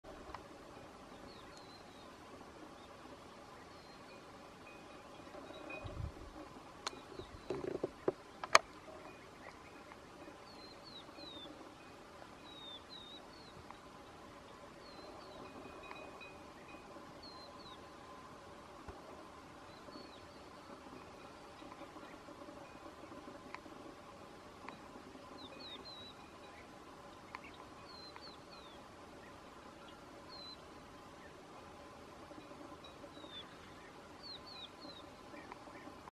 Falco Lanario / Parco Naz. Alta Murgia / fine Luglio 2017
Esemplare di Falco Lanario in sosta sulle pietraie carsiche
all' interno del Parco Nazionale dell' Alta Murgia .
23 Luglio 2017... il territorio fu
devastato da incendi dolosi distruttivi.